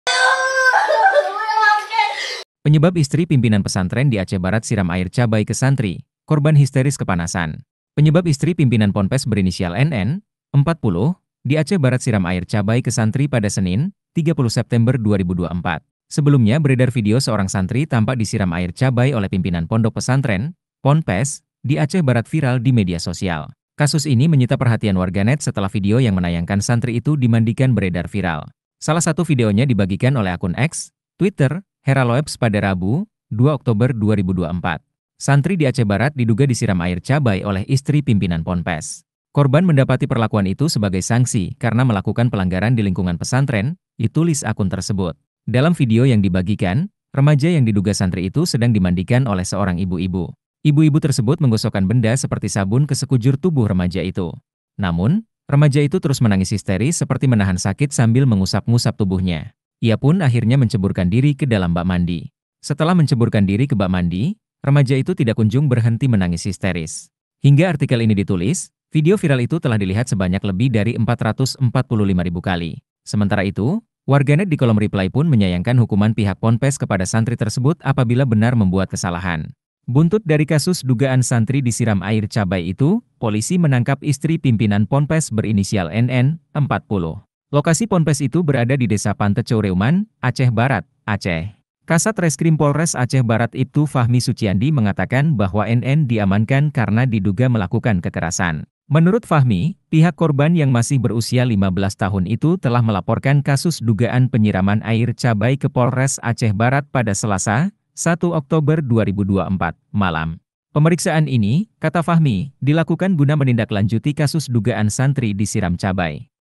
Penyebab Istri Pimpinan Pesantren di Aceh Barat Siram Air Cabai ke Santri, Korban Histeris Kepanasan
Penyebab istri pimpinan ponpes berinisial NN (40) di Aceh Barat siram air cabai ke santri pada Senin (30/9/2024).

Editor: Muhammad Maulana Ahmad Al H